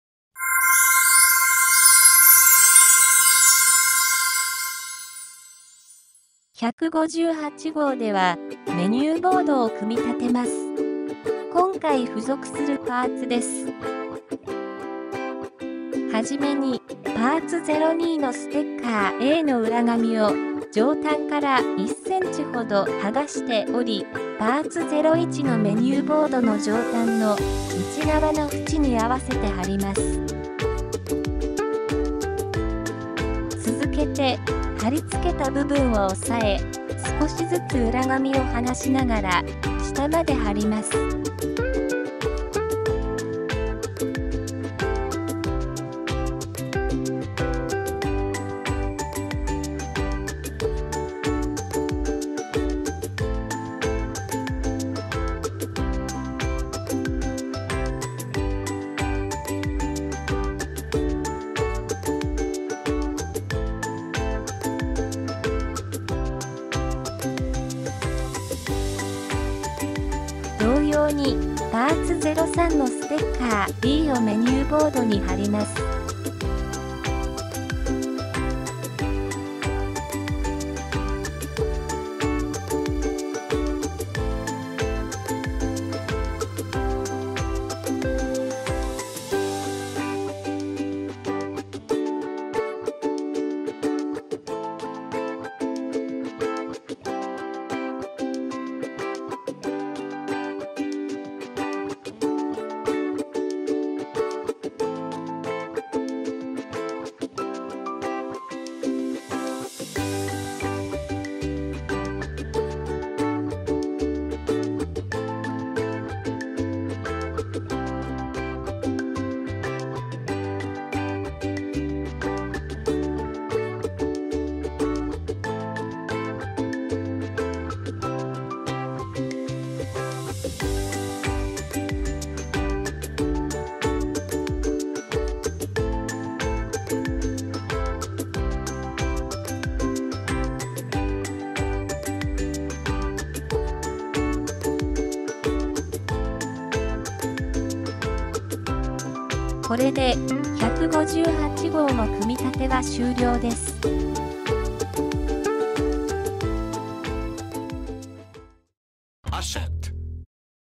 ディズニードールハウスをつくる 組立動画１５８号
158号では、メニューボードの両面にステッカーを貼ります。